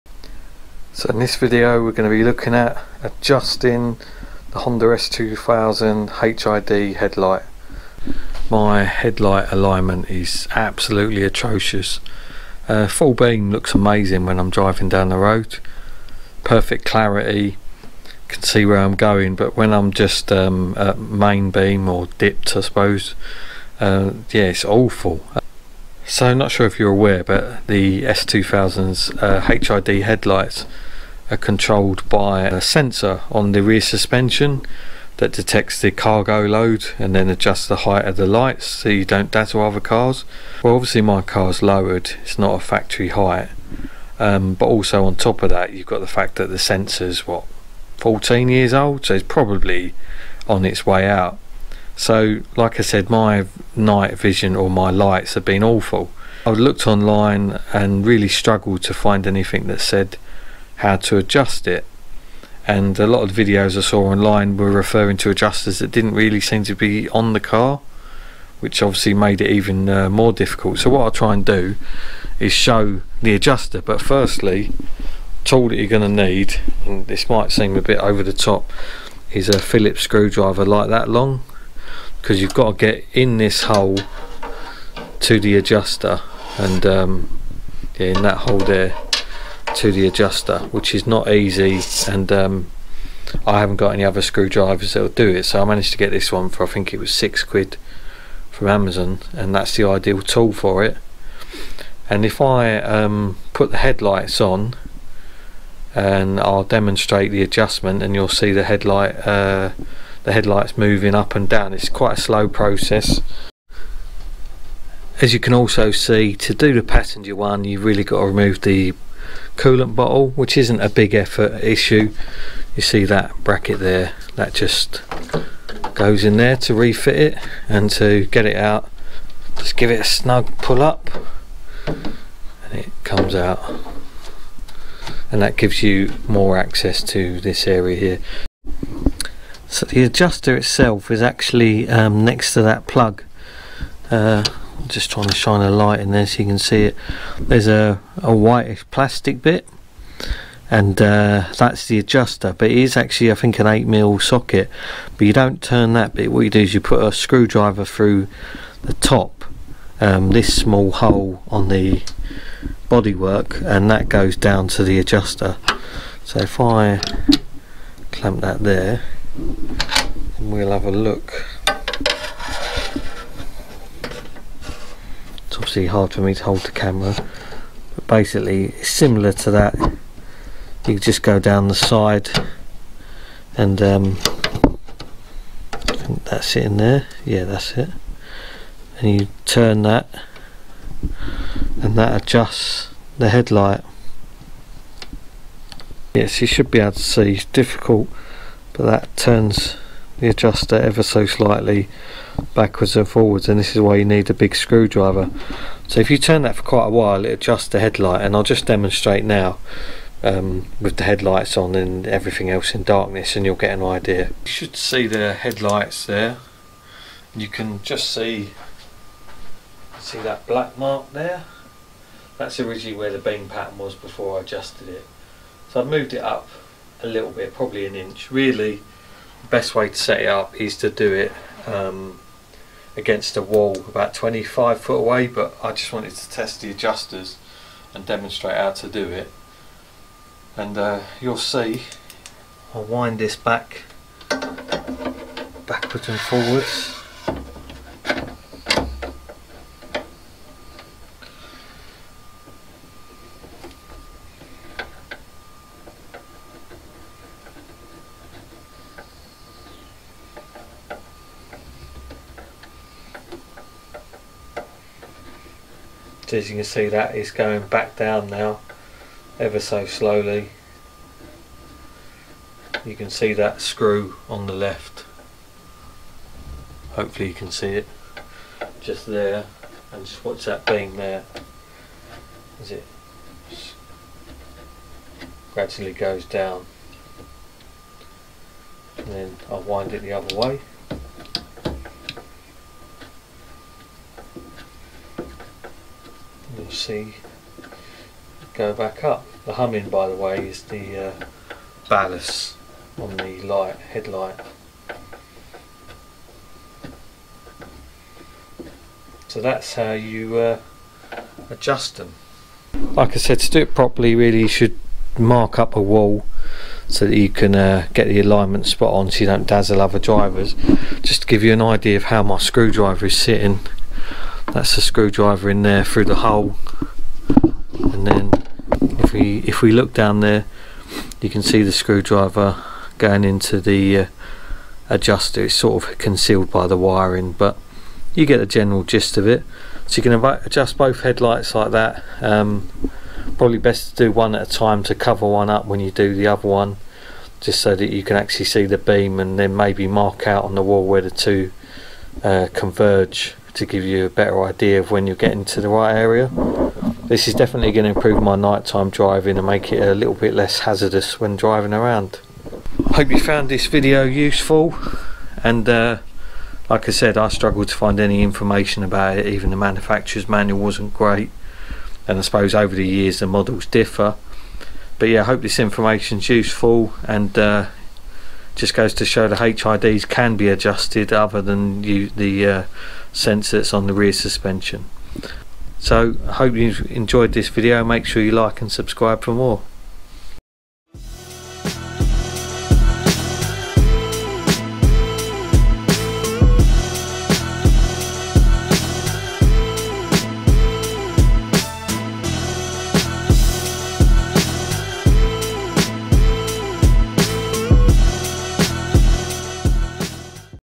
HONDA S2000 HID Headlight adjustment
On the UK s2000 the HID headlight height is controlled by motors and a sensor mounted on the area suspension. Based on vehicle load and other factors it chooses the "correct" height so that you dont dazzle other drivers. The issue with this is that my cars lights are pointing too far down on the ground and this called for some adjustment. I struggle to find a decent video or write up detailing the adjustment and how to get it to. I thought it would be worth sharing the information for anyone else who needs to adjust their headlights. Of course make sure you adjust them sensible to not blind other drivers. This video is purely to show you how to adjust the lights not how to set them up.